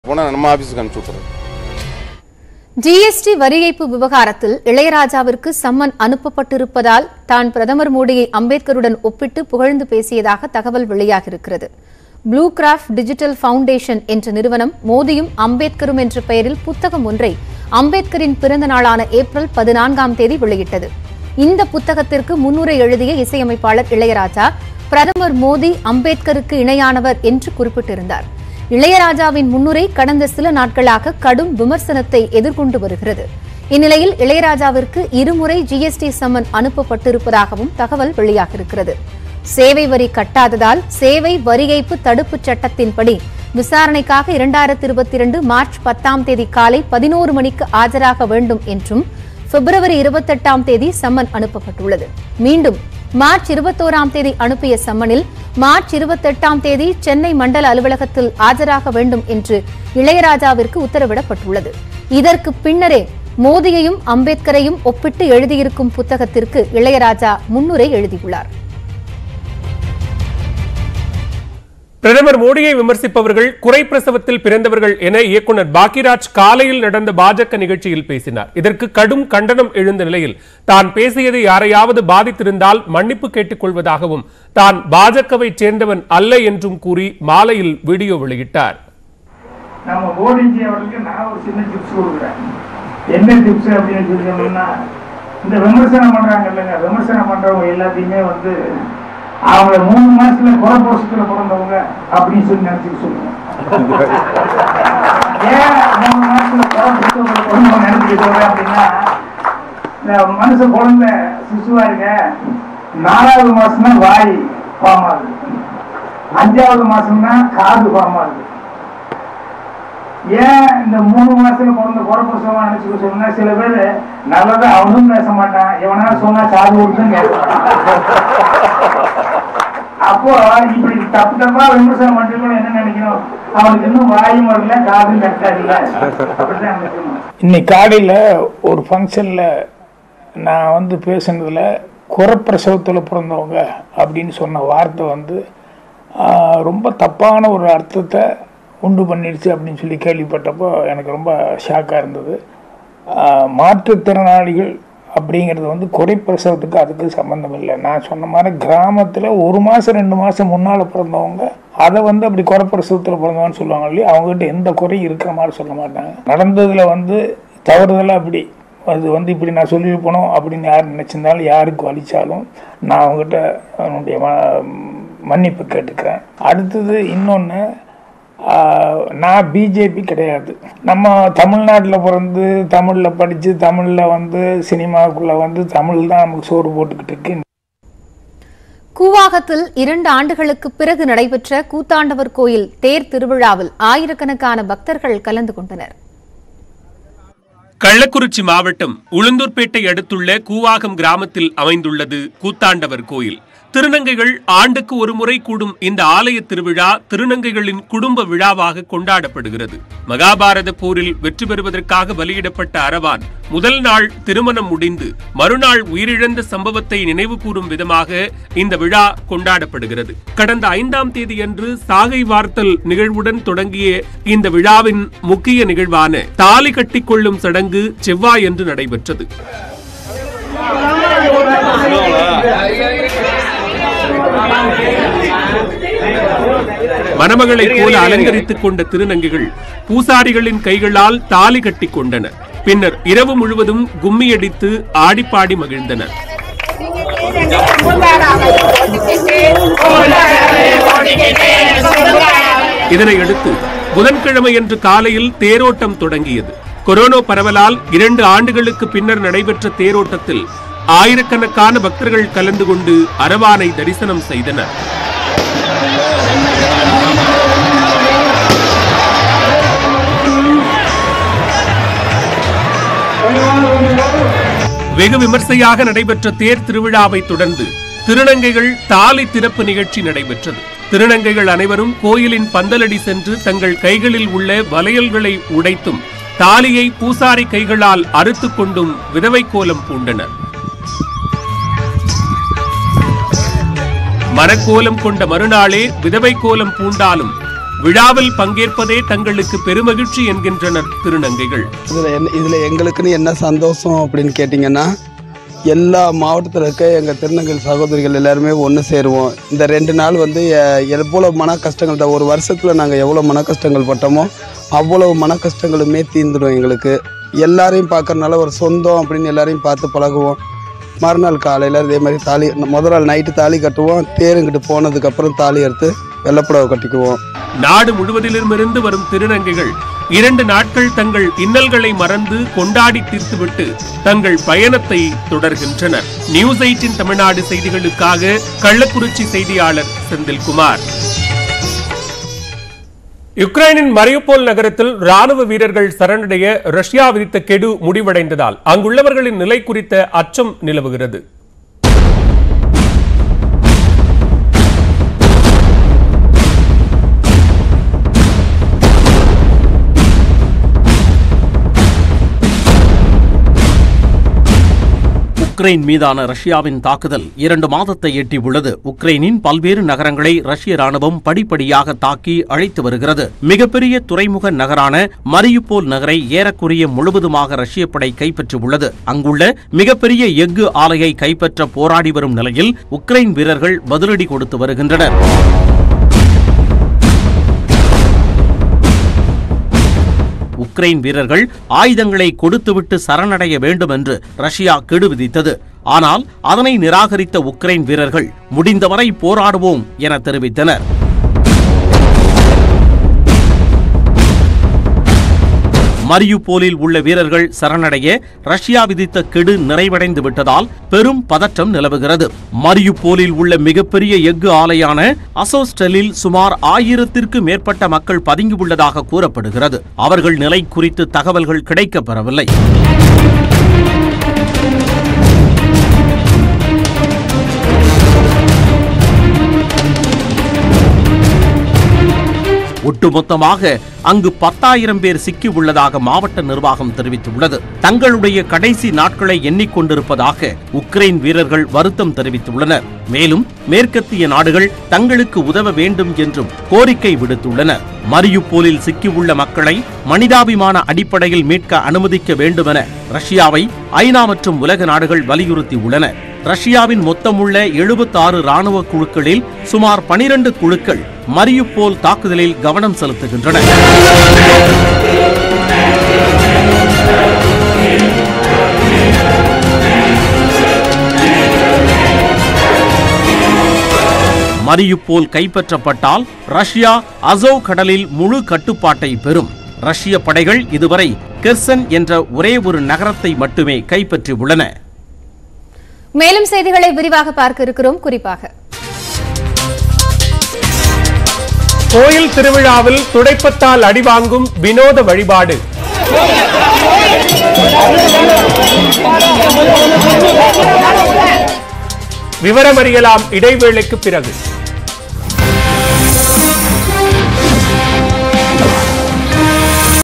Ilayaraja GST வரி ஏய்ப்பு விவகாரம் - இளையராஜாவுக்கு 2 முறை சம்மன்
Ilayaraja GST வரி ஏய்ப்பு விவகாரம் - இளையராஜாவுக்கு 2 முறை GST சம்மன் அனுப்பப்பட்டுள்ளதாக தகவல் வெளியாகியுள்ளது